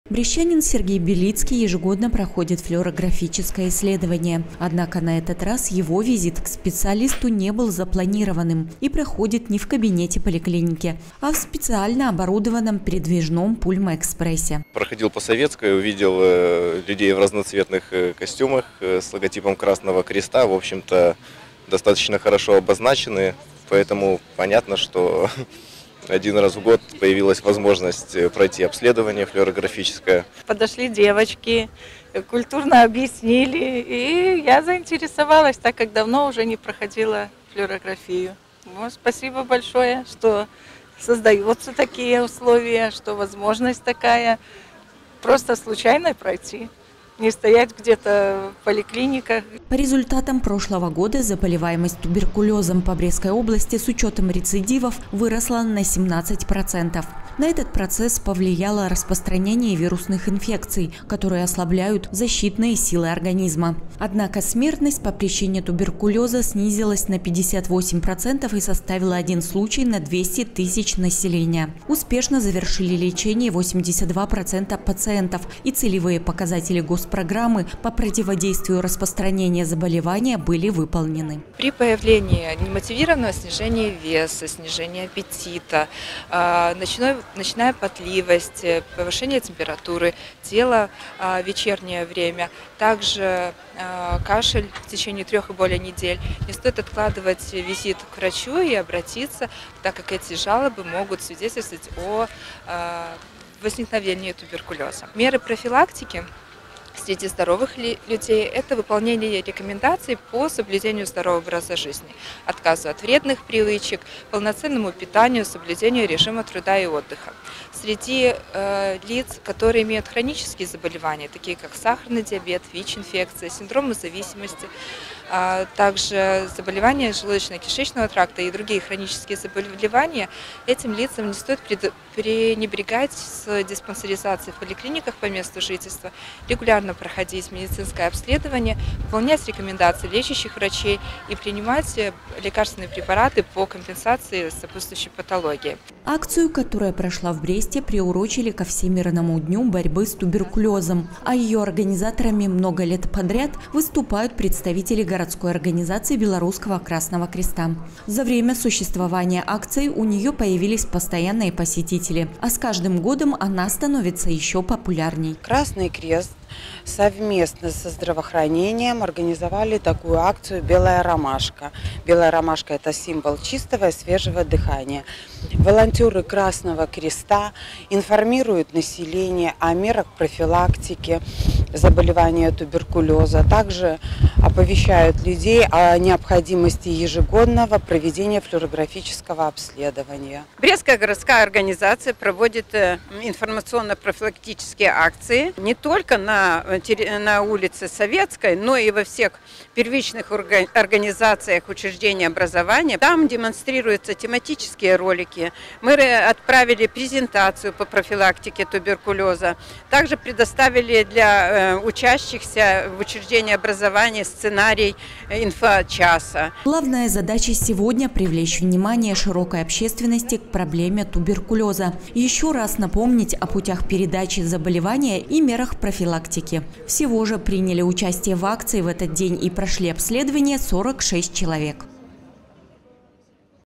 2023-03-24 г. Брест. День борьбы с туберкулёзом. Новости на Буг-ТВ.
Сегодня во всем мире отмечается День борьбы с туберкулёзом. Несмотря на положительную динамику эпидемических показателей, данное заболевание в Беларуси все еще продолжает наносить значительный ущерб здоровью населения остаётся важной социальной проблемой.  Чтобы максимально информировать людей о профилактике и лечении этого недуга, Брестская городская организация Белорусского общества Красного Креста ежегодно 24 марта организует акцию. В центре города каждый желающий в этот день может быстро и бесплатно пройти флюорографическое обследование в специально оборудованном автомобиле.